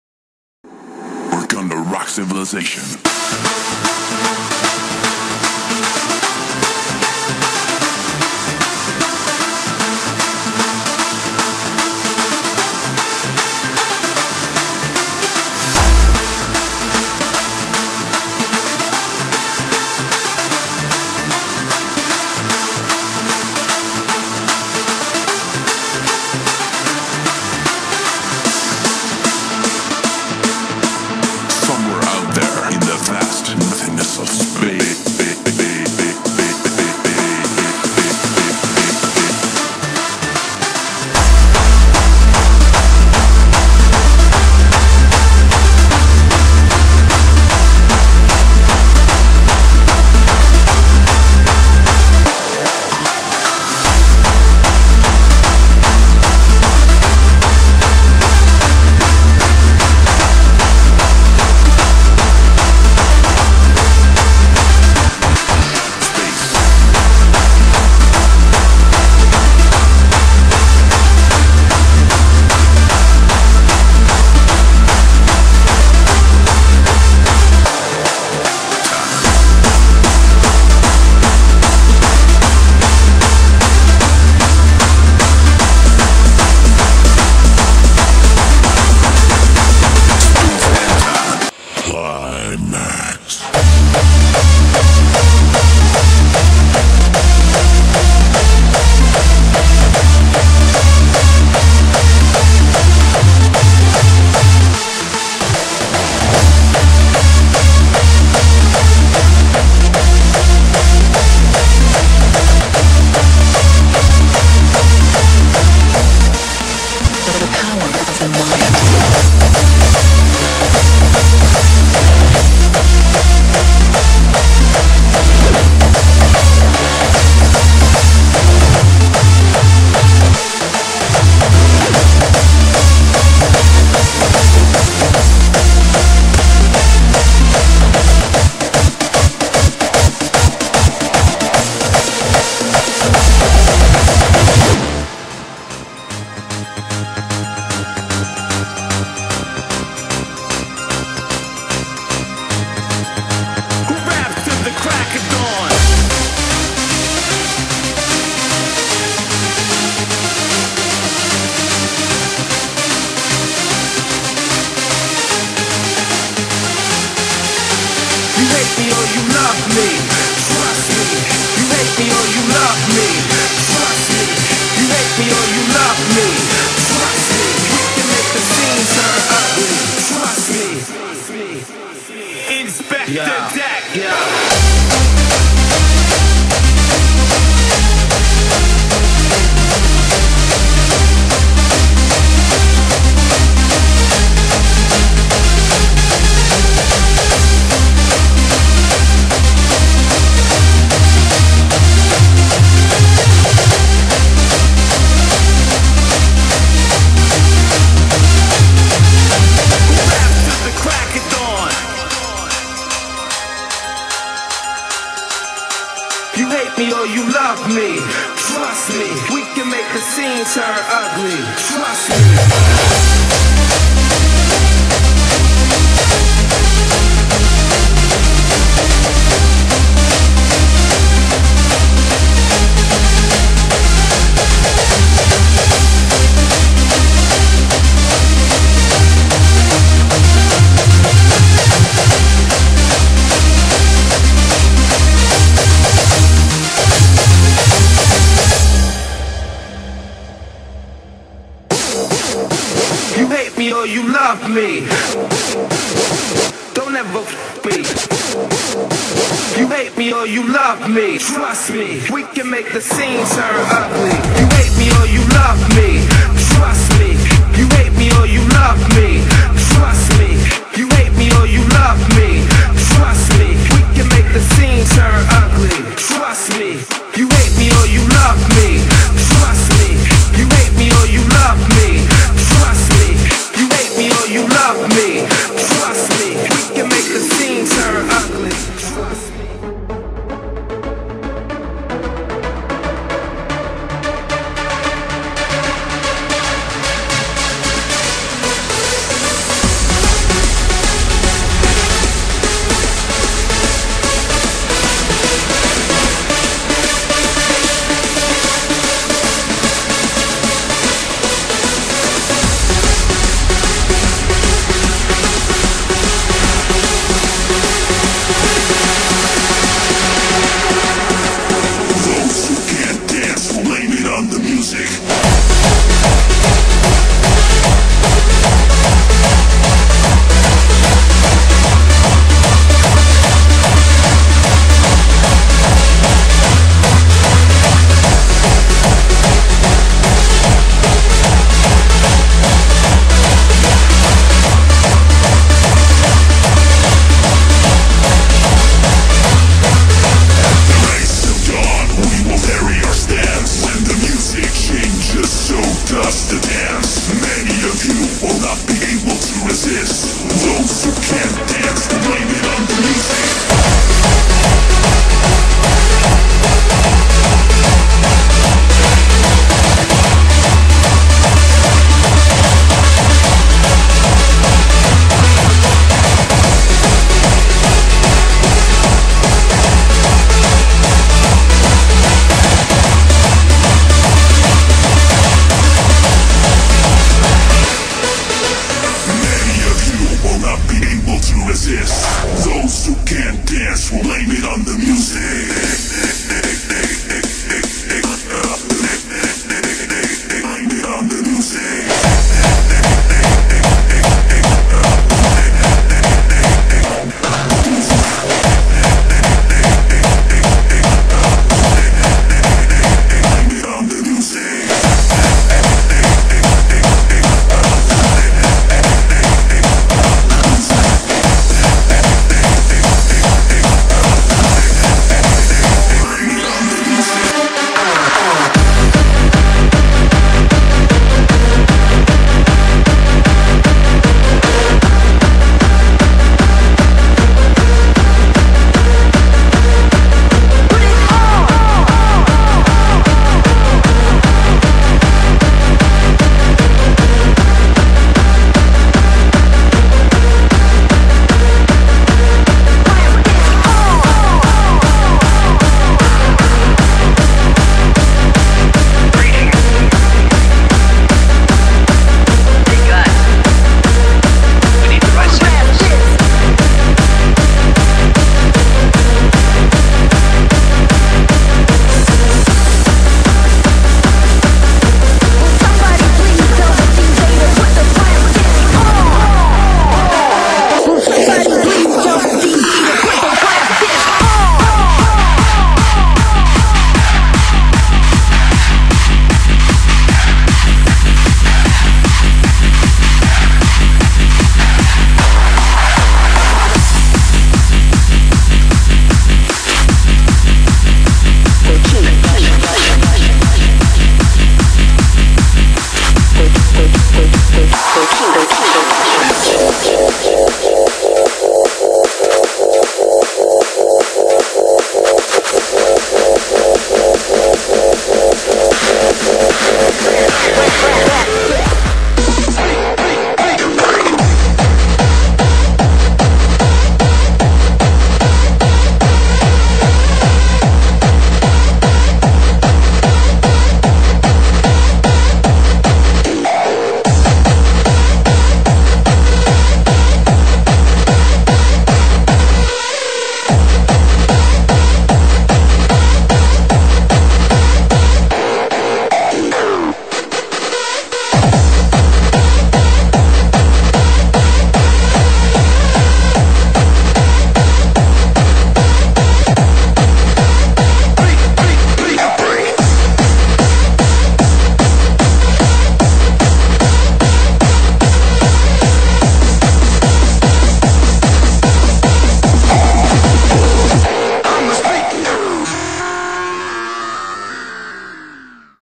Over 9 MIN Headhunterz-MIX !
After my last BAD Video, now my very better new Video is online ! Listen and enjoy the feeling of Headhunterz. Notes and 5* are allowed ^^.